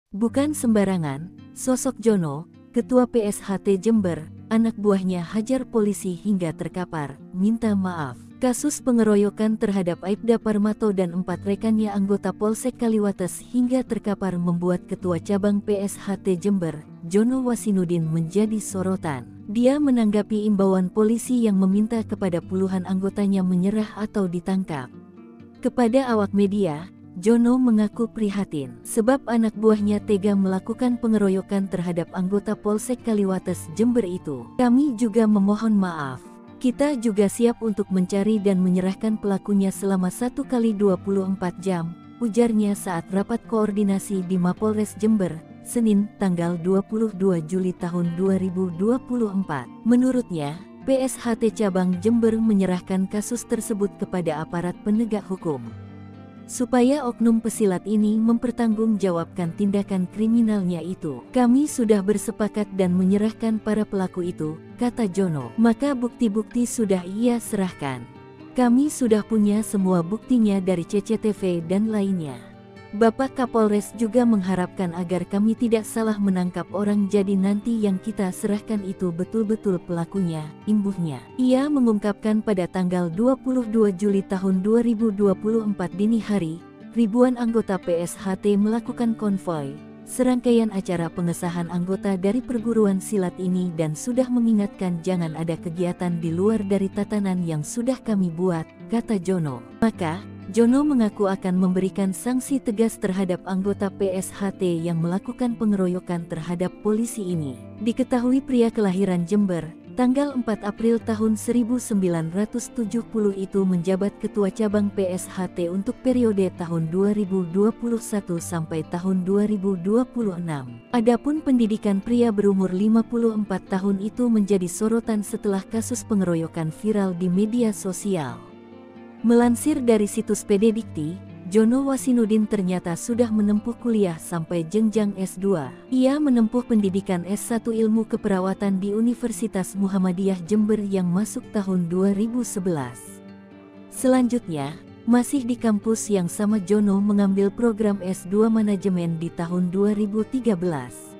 Bukan Sembarangan, SOSOK JONO, Ketua PSHT Jember, Anak Buahnya Hajar Polisi Hingga Terkapar, Minta M
KASUS pengeroyokan terhadap Aipda Parmato dan 4 rekannya anggota Polsek Kaliwates hingga terkapar membuat Ketua Cabang PSHT Jember, Jono Wasinudin menjadi sorotan.

Dia menanggapi imbauan polisi yang meminta kepada puluhan anggotanya menyerah atau ditangkap. Kepada awak media, Jono mengaku prihatin.

Sebab anak buahnya tega melakukan pengeroyokan terhadap Anggota Polsek Kaliwates Jember itu.

Lihat informasi terkini bersama kami, di:

Palembang.tribunnews.com

Sumber Video: Tribunnews
Editor Video: Aldi
Naskah: Hendra